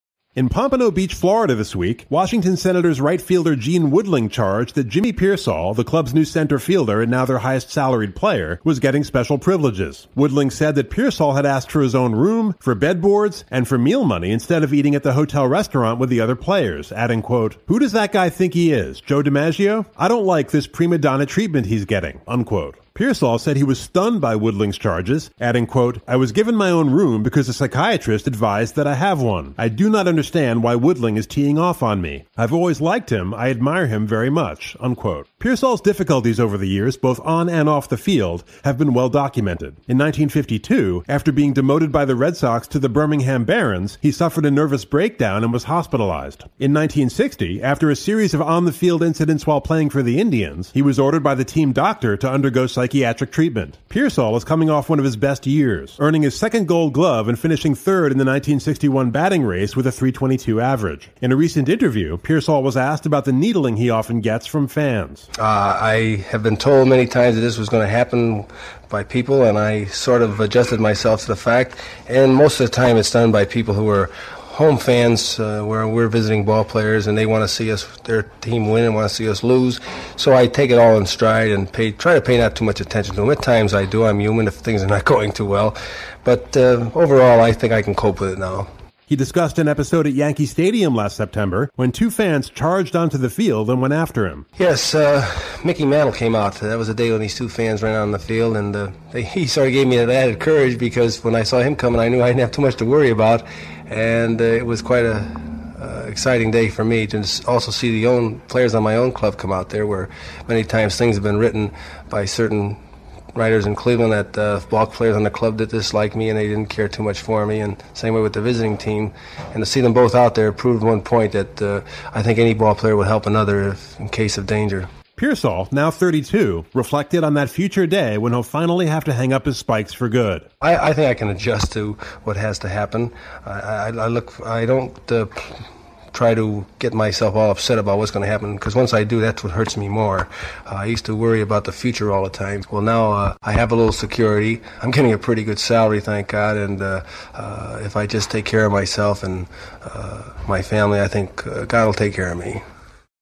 Mar. 7, 1962 - Outfielder Jimmy Piersall Speaks Out
Mar. 7, 1962 - Gene Woodling, Washington’s leading hitter and the American League player representative, charged this week that Jimmy Piersall, the Senators’ highest-salaried player, was receiving special privileges from the club. “Who does that guy think he is — Joe DiMaggio?” asked Woodling. “I don’t like this prima donna treatment he’s getting. It isn’t fair to the other players. They’re human, too.” Woodling said other players on the team had complained that Piersall had asked for special privileges, such as rooming by himself, bed boards, and demanding meal money in lieu of eating at the hotel restaurant with the other players. Piersall was stunned by Woodling’s charges. “I never asked for special privileges and I never will,” he said. “I was given a special room because a psychiatrist advised that I have one. I don’t like noise. I am an early riser. I don’t understand why Woodling is teeing off on me. I have always liked him. I admire him very much.” Ed Doherty, general manager of the Senators, denied Woodling’s charges. He acknowledged that Piersall roomed alone, but only because “he’s high-strung and would benefit by privacy.” “Sure, Jimmy asked for bed boards. What’s so bad about that? He has a bad back and the boards make him more comfortable. I ask for them myself at every hotel. As for Jimmy asking for meal money, the first night he was here he was not satisfied with the food, and he asked for money to eat out. I told him it couldn’t be done. The only players we permit to eat out at the club’s expense are married men living off the base. They are permitted $12 a day for meals. Jimmy didn’t argue. Matter of fact, he likes the food at the hotel now.”